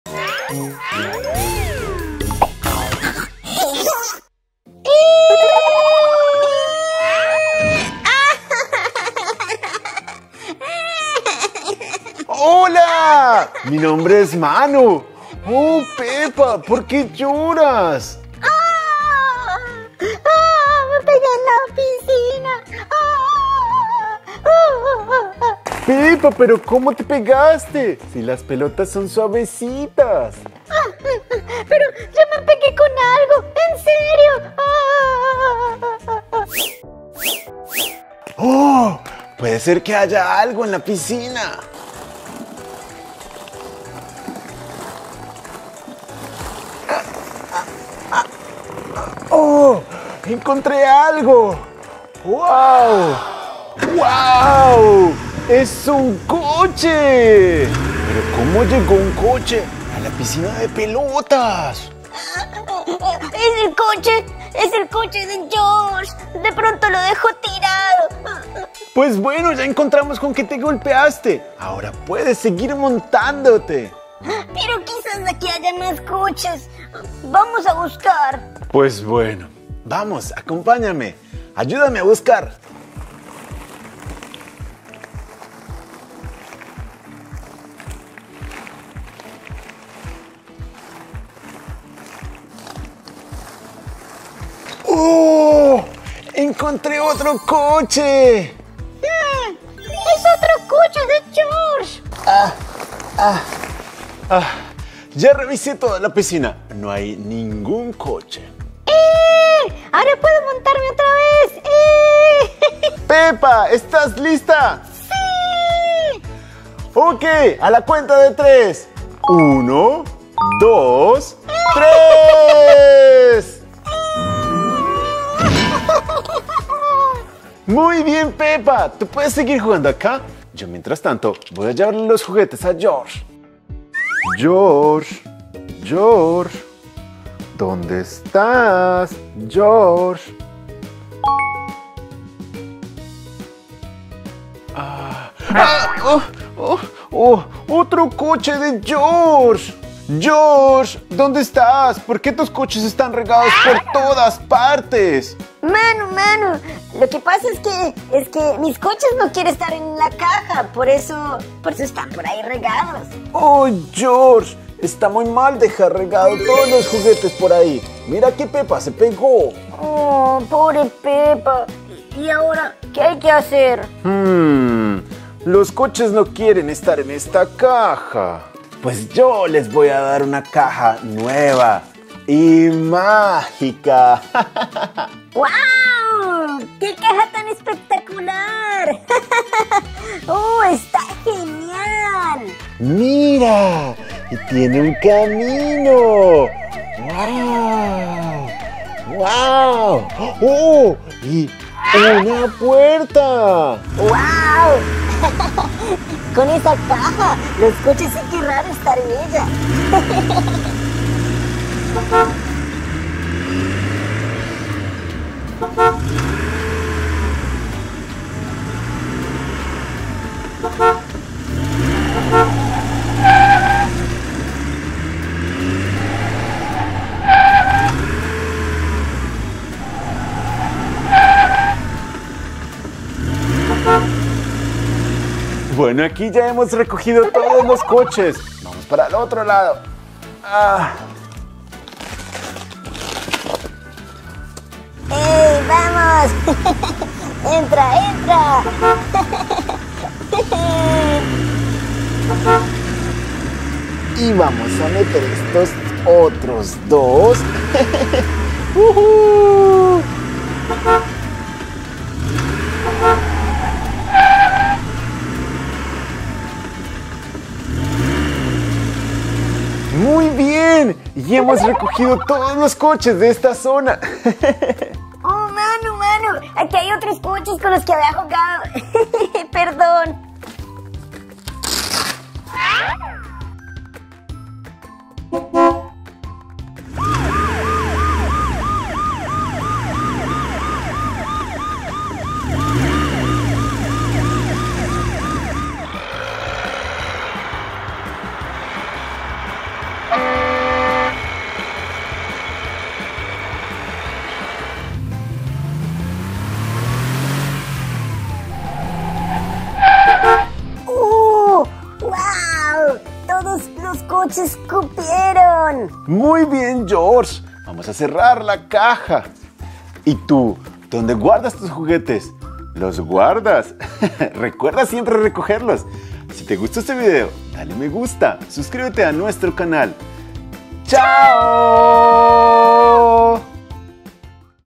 Los coches de George buscan un hogar. Juguetes de peluche para niños
Juguetes de peluche para niños. Mira los vídeos con Peppa, George y sus coches en el canal "Juguetes de Peluche". Peppa está jugando en la piscina de bolas, subiéndose al tobogán y de repente... ¡golpea algo! ¿Averiguamos qué podría haber golpeado? ¡Miremos!... ¡Estos son los coches de George! Habría que ponerlos en su lugar...